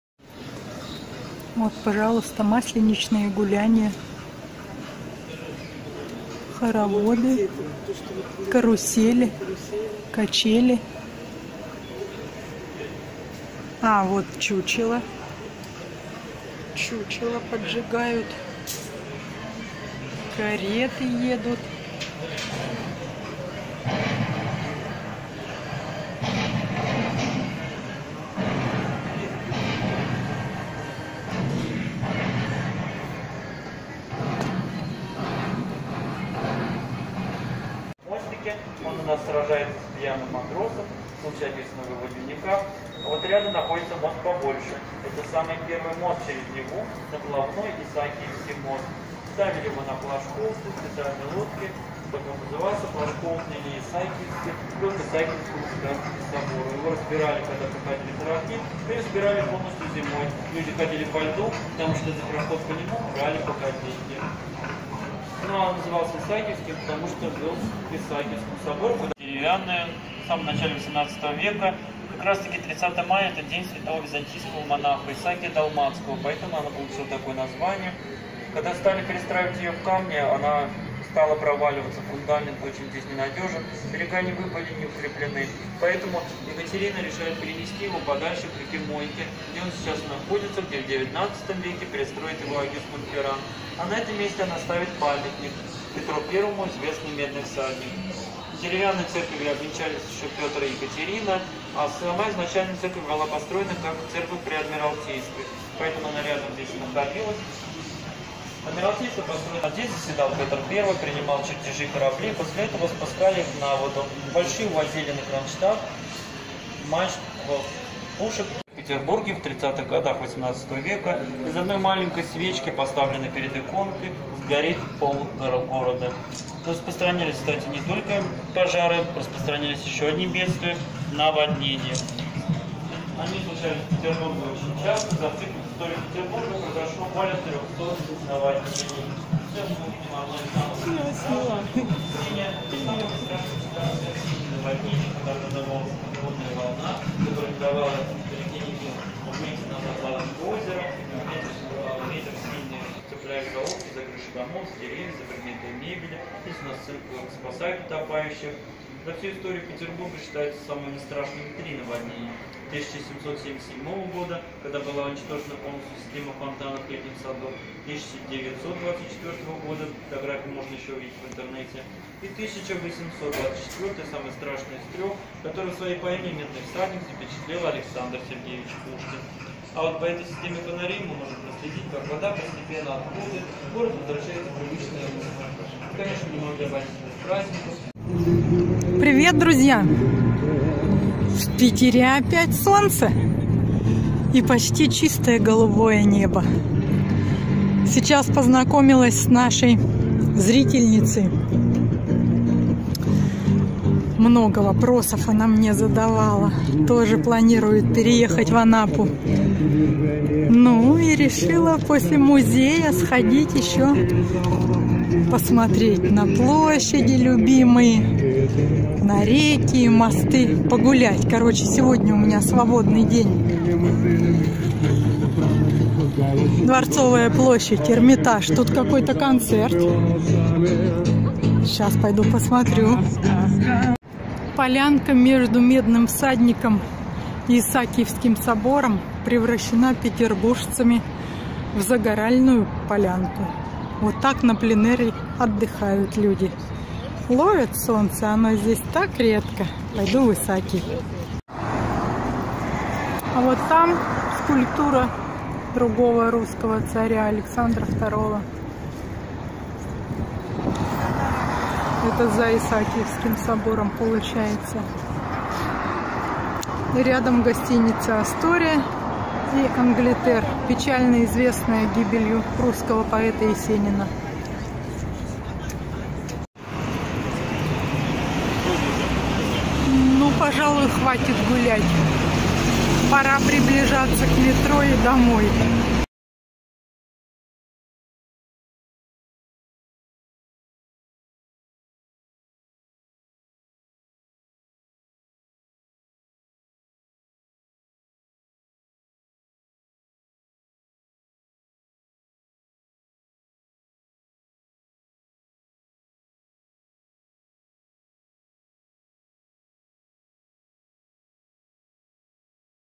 Прогулка по Питеру и музей-макет «Петровская Акватория» 11.05.2018
Музей открылся в сентябре 2014 года. На территории площадью 500 м² разместились все знаковые дворцово-парковые ансамбли, крепости, здания и пригороды Петербурга XVIII века.
авторы канала Дмитрий и Оксана Сагара